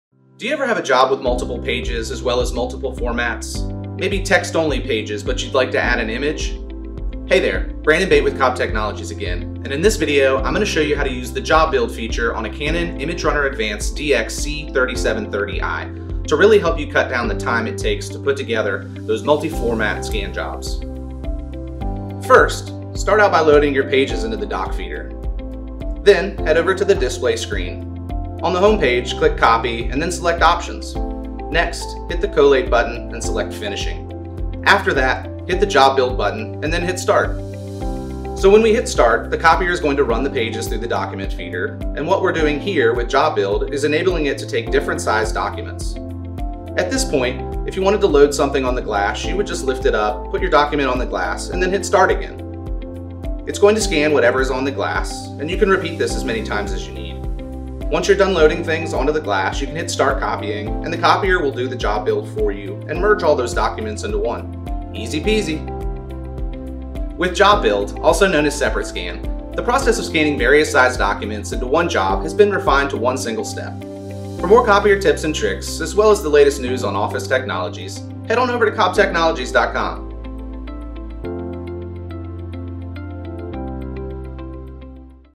How to Use the Job Build Feature
In this tutorial, we'll explain how to use the job build feature on your Canon DX ImageRUNNER C3730i.

For more information about Cobb Technologies and our office technology solutions, visit our